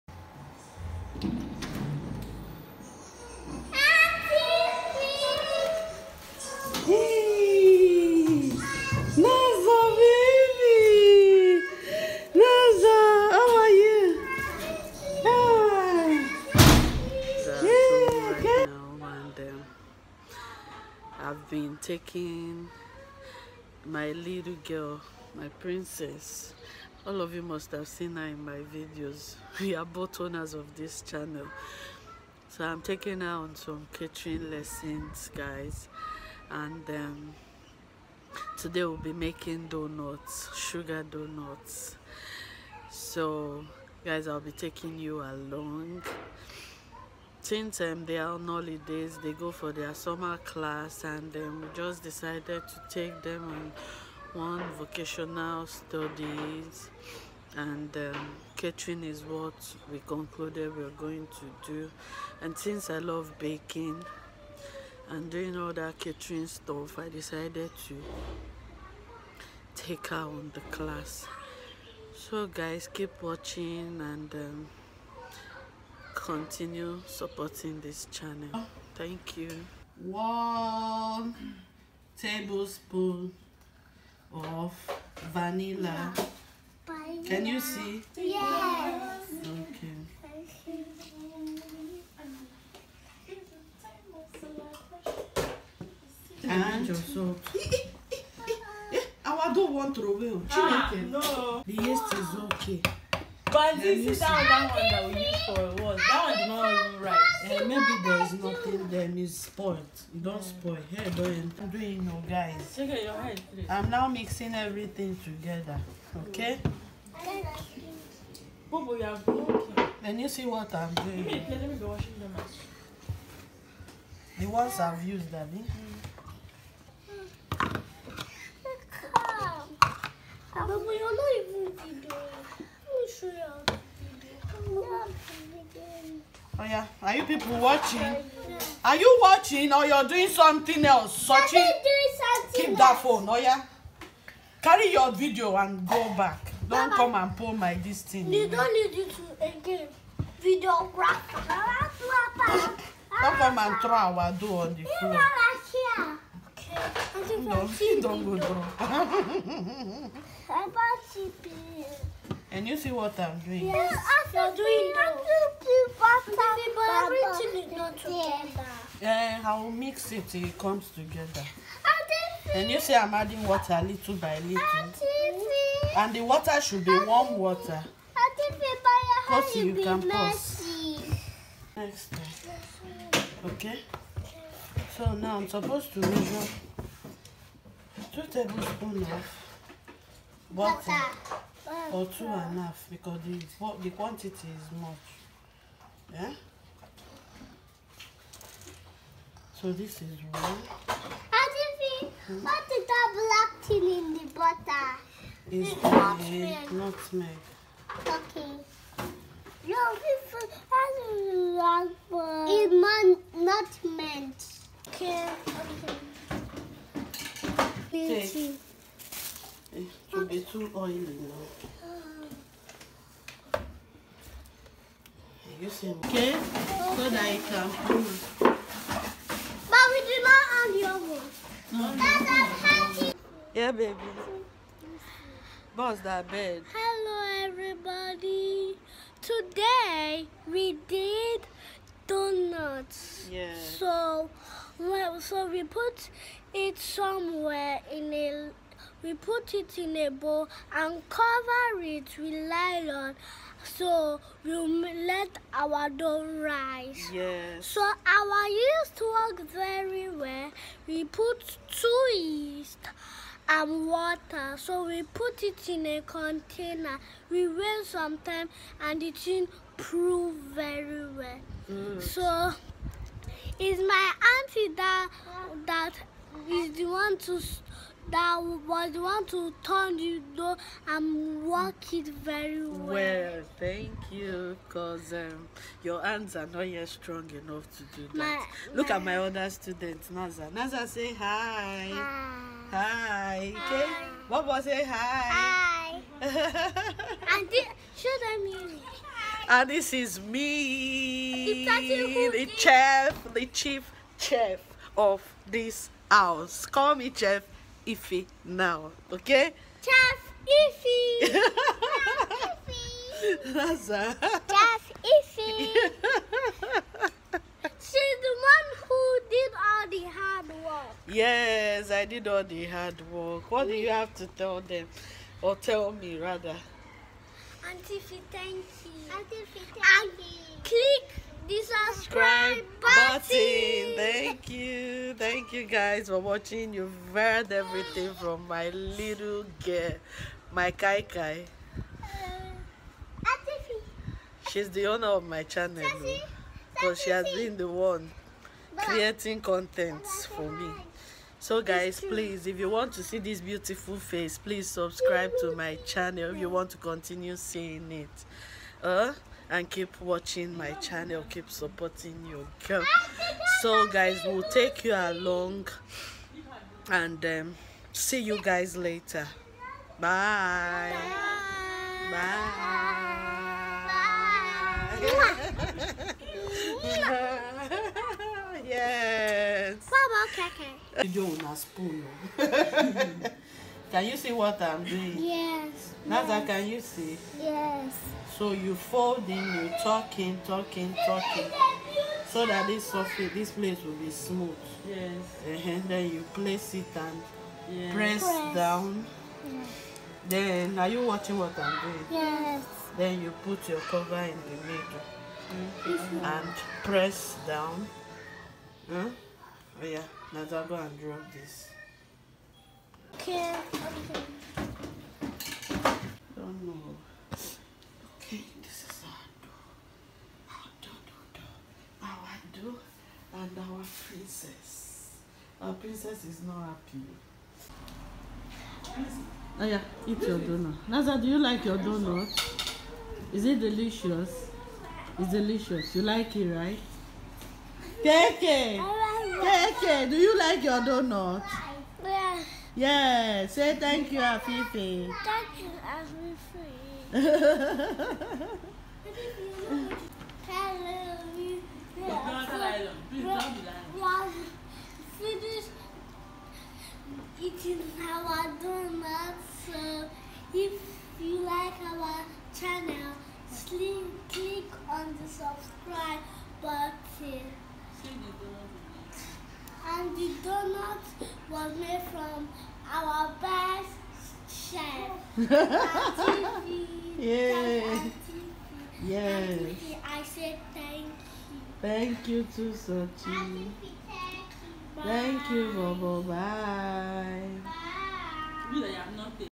Summer holiday vlog1+Teaching the kids how to make the yummiest doughnut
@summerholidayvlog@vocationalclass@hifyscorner
Watch me teach the kids how to mk doughnuts,I promise u will enjoy the video.